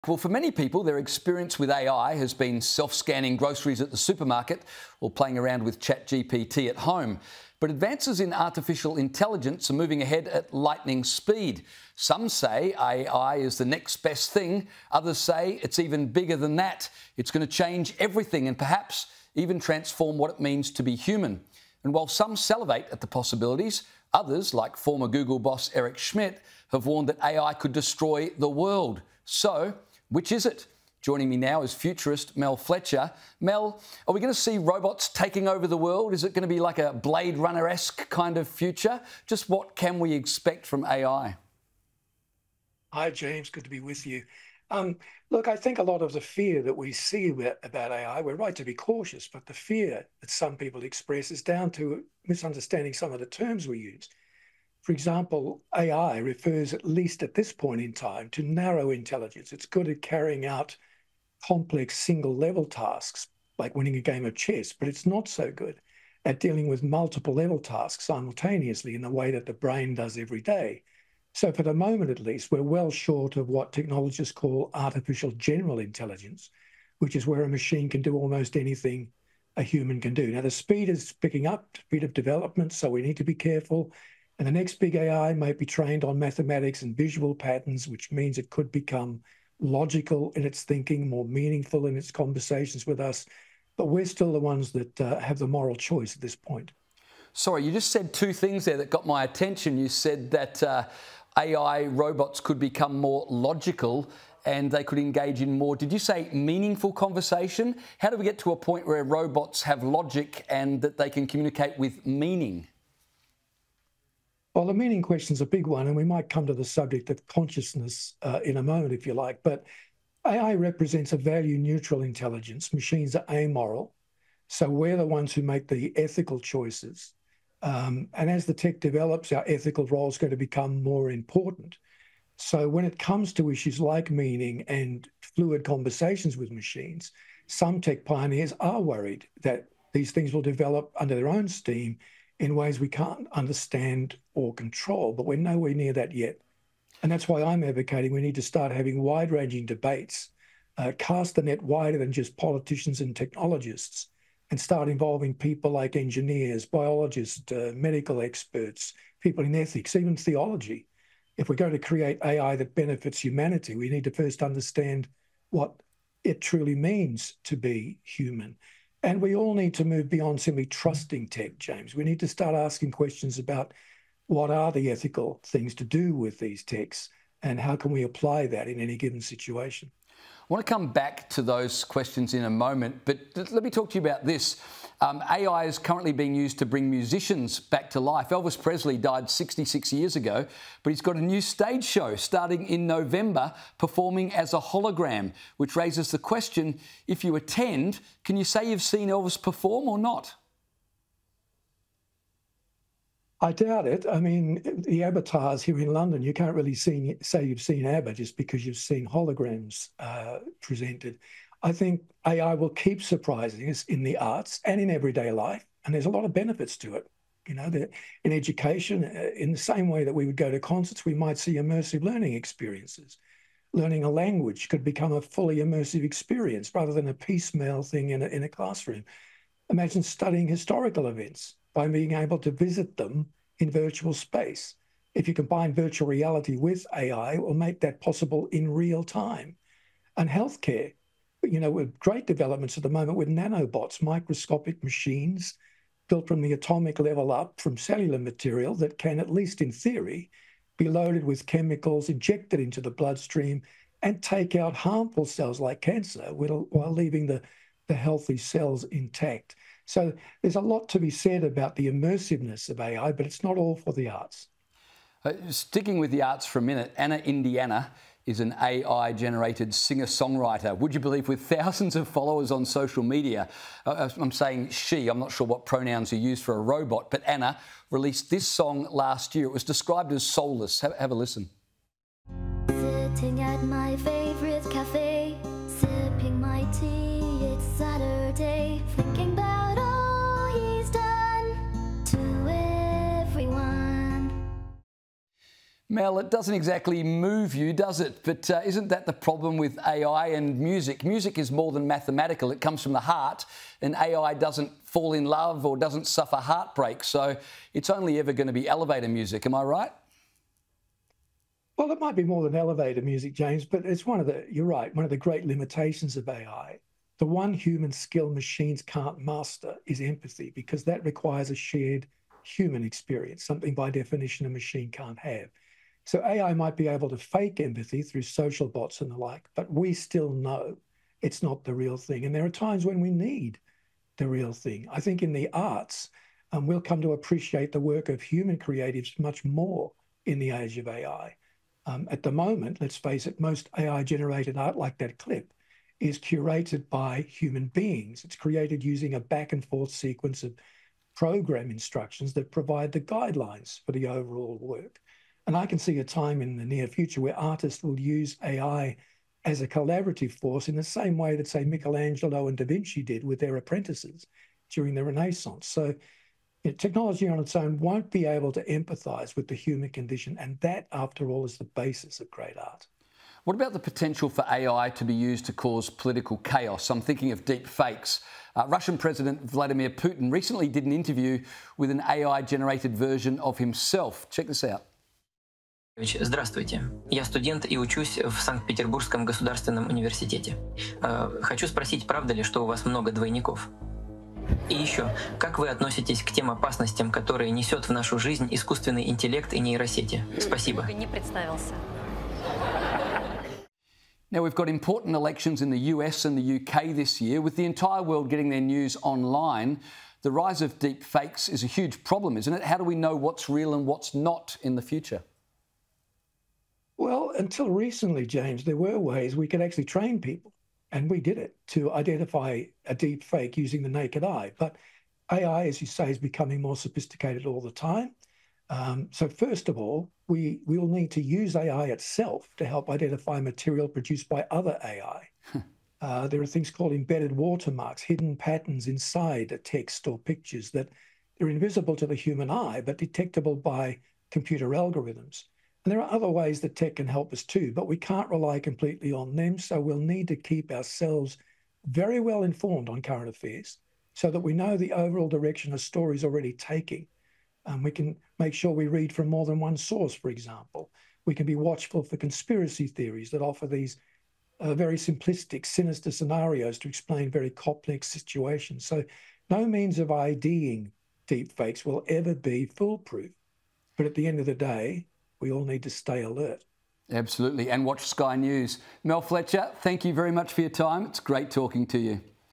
‘A lot of the fear’ about AI comes from a ‘misunderstanding’ of terms used to describe it
Futurist Mal Fletcher says “a lot of the fear” about artificial intelligence comes down to a “misunderstanding” of some of the terms used to describe it.

For many people, their experience with artificial intelligence has been self-scanning groceries at the supermarket or playing around with ChatGPT at home, but advances in artificial intelligence are moving ahead at lightning speed.

While some are excited about the possibilities, others, like former Google boss Eric Schmidt, have warned that AI could destroy the world.

“I think a lot of the fear that we see about AI – we’re right to be cautious – but the fear that some people express is down to misunderstanding some of the terms we use,” Mr Fletcher told Sky News Australia host James Macpherson.

“For example, AI refers, at least at this point in time, to narrow intelligence. It is good at carrying out complex single-level tasks, like winning a game of chess, but it’s not so good at dealing with multiple-level tasks simultaneously in a way that the brain does every day.